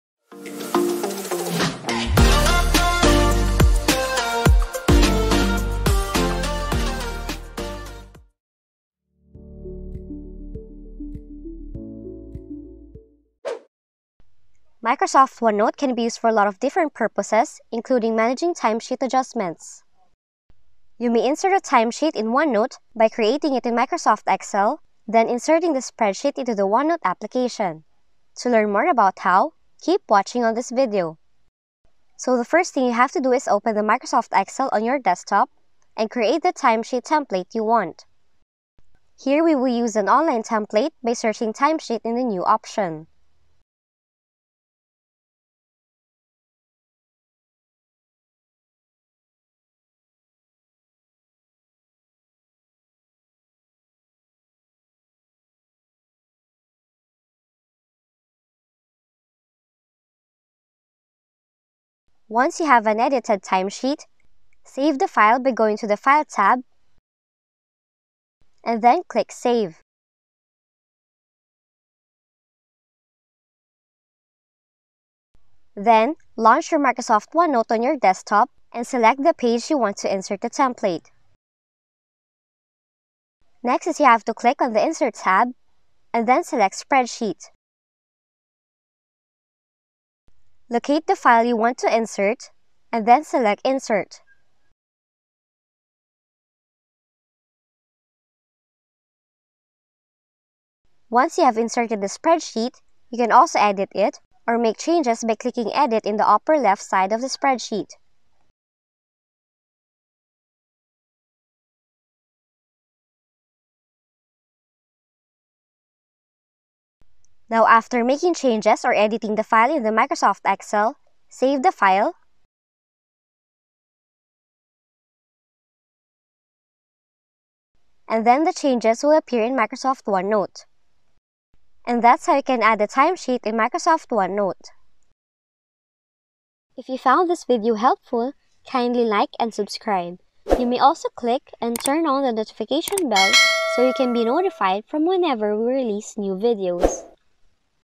OneNote - How to Add a TIMESHEET (Using a Free Template)
--- Would you like to have a Timesheet right inside of OneNote? Fortunately, there's an easy way to insert one into a OneNote page. Watch this tutorial to see exactly how! ---